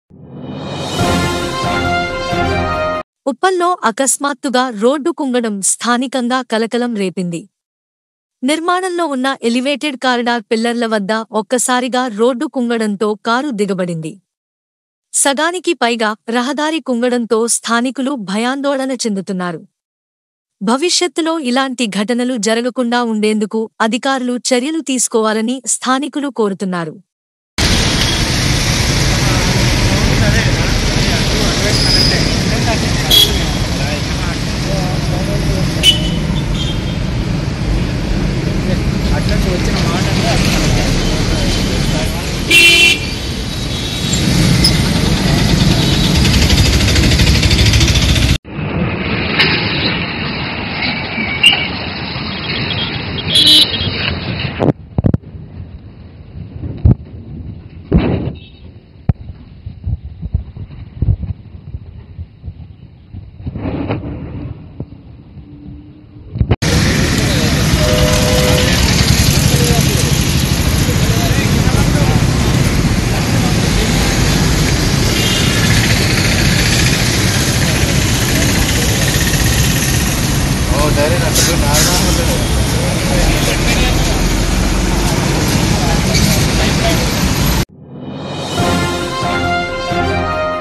ఉప్పల్ లో కుంగిన రోడ్డు.. | Road Collapsed In Uppal | Uppal Latest News | FM News
ఉప్పల్‌లో అకస్మాత్తుగా రోడ్డు కుంగడం స్థానికంగా కలకలం రేపింది. నిర్మాణంలో ఉన్న ఎలివేటెడ్ కారిడార్ పిల్లర్ల వద్ద ఒక్కసారిగా రోడ్డు కుంగడంతో కారు దిగబడింది. సగానికి పైగా రహదారి కుంగడంతో స్థానికులు భయాందోళన చెందుతున్నారు.  భవిష్యత్తులో ఇలాంటి ఘటనలు జరగకుండా ఉండేందుకు అదికారులు చర్యలు తీసుకోవాలని స్థానికులు  కోరుతున్నారు